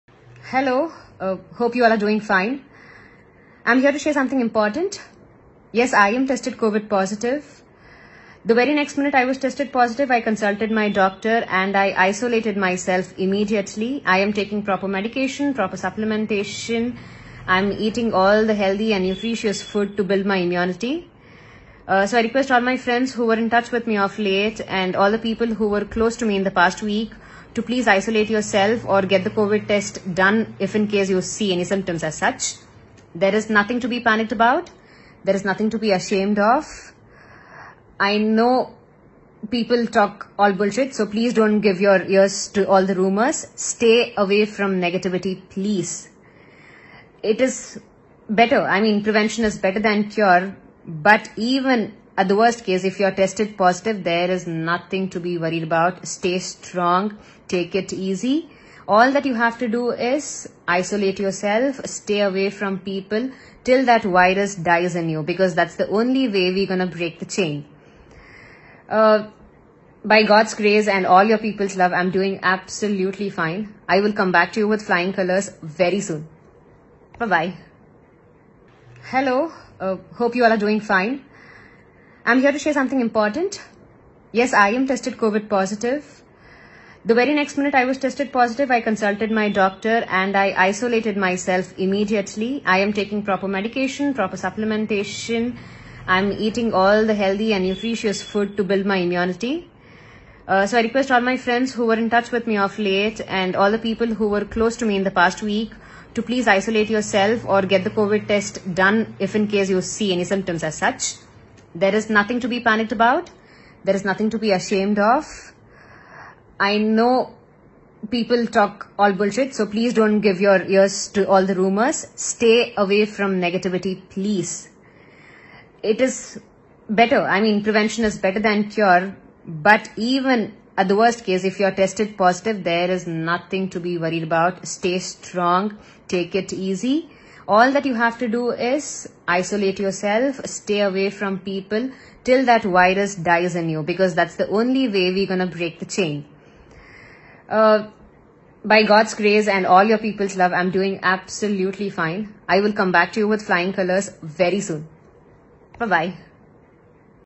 Serial Actress Navya Swamy Tested Positive | Navya Swamy Gave Clarity About Her Health |Yellow Pixel
Watch  Serial Actress Navya Swamy Tested Corona Positive | Navya Swamy says there is nothing to fear even if you test COVID19 positive | Yellow Pixel

TV Actress Navya Swamy Gave Clarity About Her Health Condition

Noted Telugu Television actress Navya Swamy who has tested positive for Covid19 has urged people to take precautions and stay away from negative.

She says there is nothing to fear even if you test COVID19 positive.

Serial Actress NavyaSwamy Tested